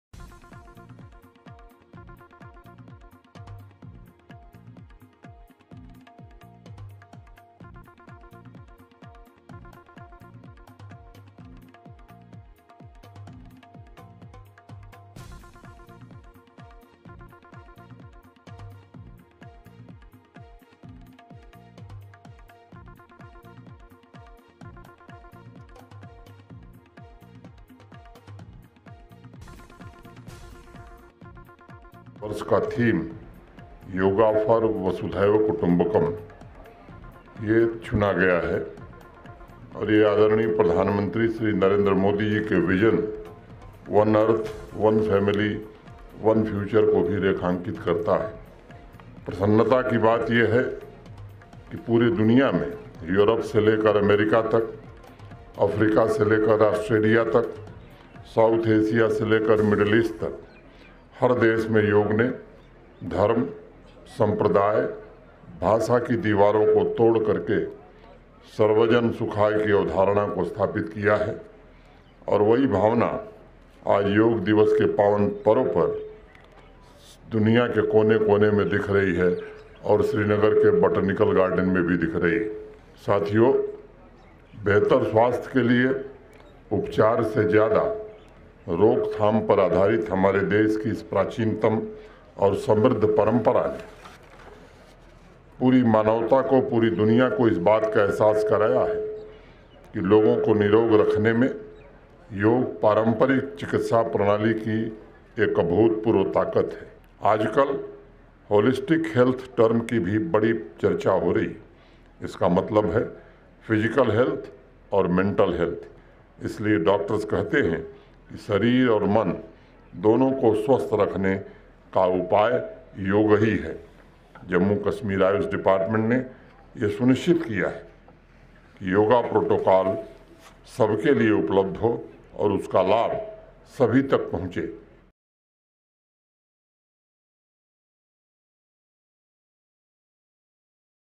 منوج سنہا نے بوٹینکل گارڈن سری نگر میں یوگا تقریب کی قیادت کی
جموں وکشمیر کے لیفٹیننٹ گورنر منوج سنہا نے نویں بین الاقوامی یوگا دن کے موقع پر بوٹینکل گارڈن میں یوگا تقریب کی قیادت کی۔انہوں نے اس موقع پر اپنے خطاب میں کہا کہ صحت مند اور تناﺅ سے پاک زندگی گذارنے کے لئے ہر کسی کو یوگا اپنے روز مرہ کے معمولات کا ایک حصہ بنانا چاہئے۔